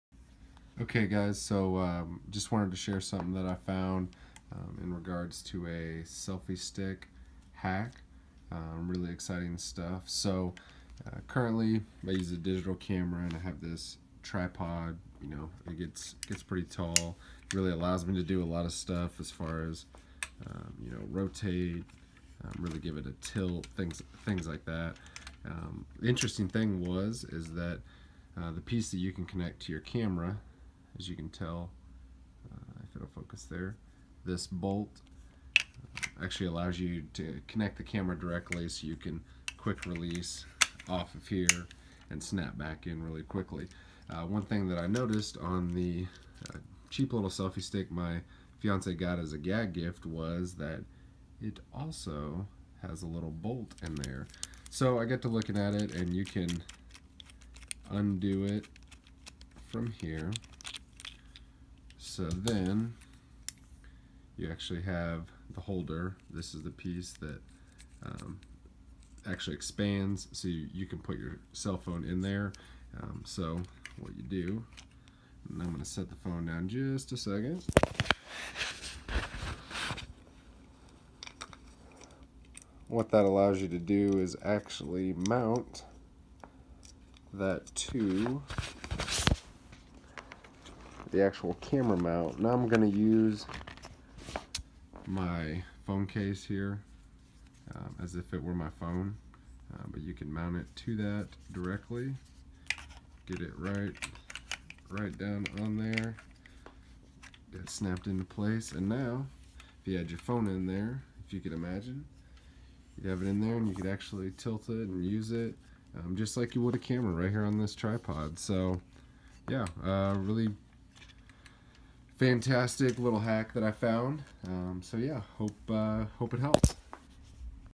Selfie stick hack HD
Found a cool way to use part of a selfie stick to hold a phone on a camera tripod.

Mono Pod - selfie stick
Sunpak 8001UT - Tripod